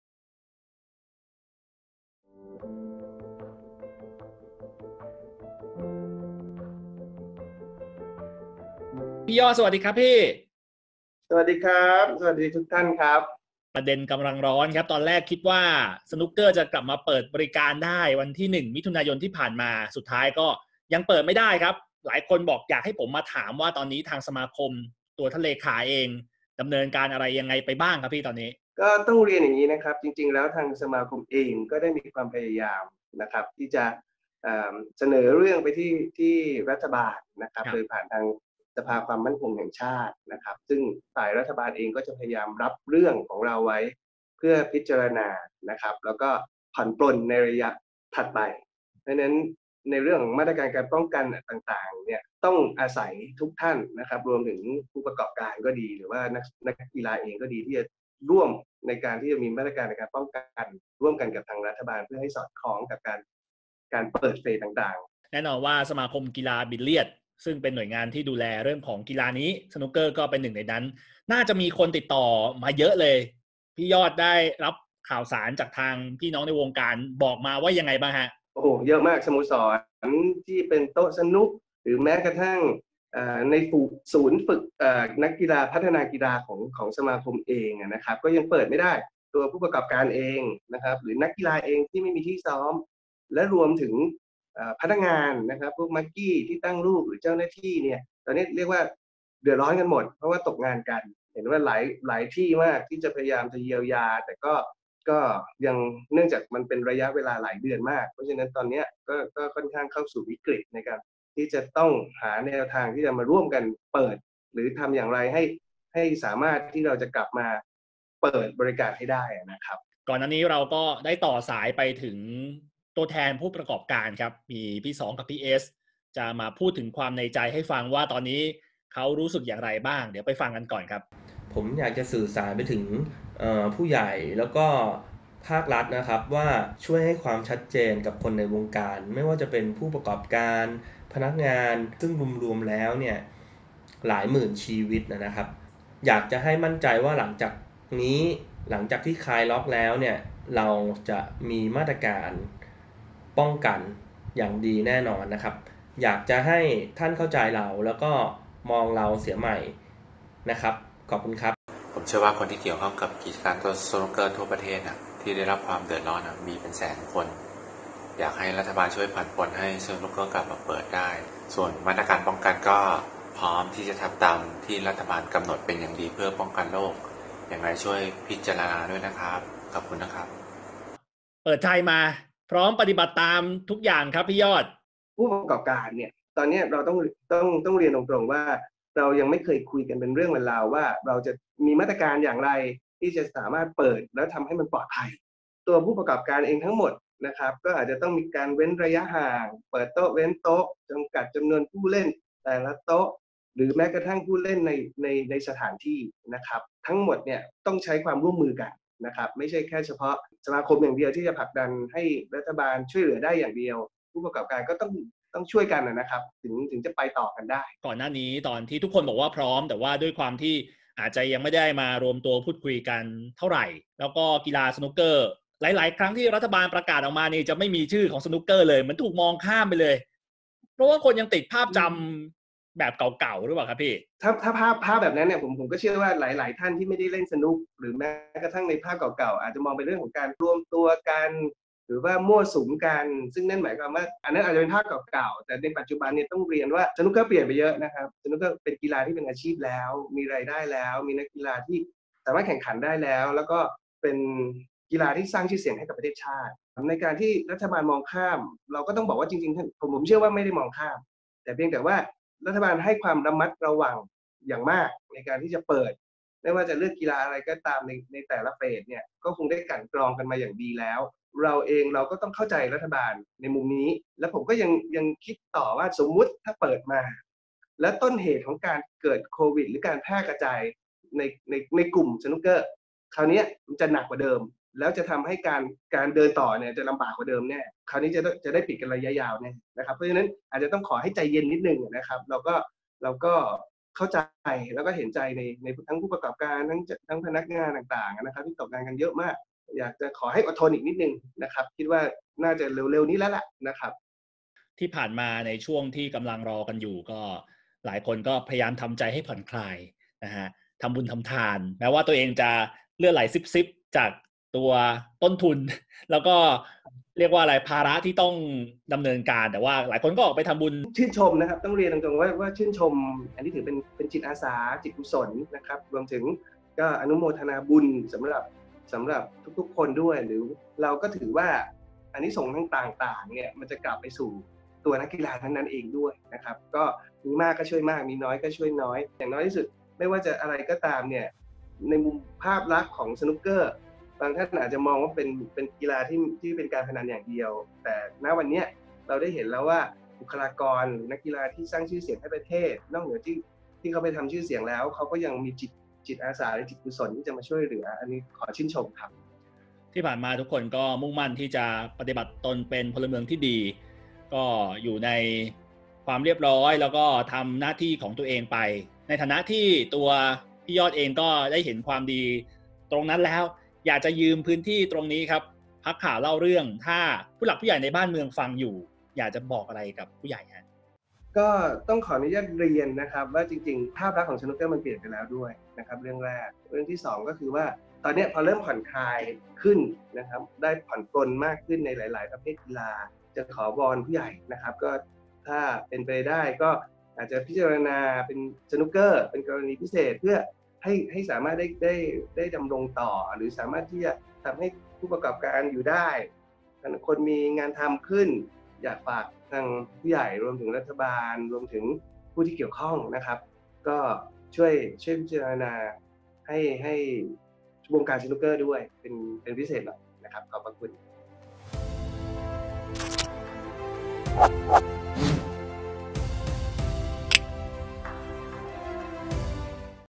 สนุกเกอร์ร้องโดนปิด 3 เดือนแทบขาดใจ
คุย “บิ๊กยอด” ประสานช่วยเหลือชาวสอยคิว โดนปิด 3 เดือนแทบขาดใจ 🇹🇭😢🎱🚫⚪️

ติดตามคลิปใหม่ ๆ ของพวกเราได้ที่

ติดตามข่าวสารต่าง ๆ เพิ่มเติมได้ที่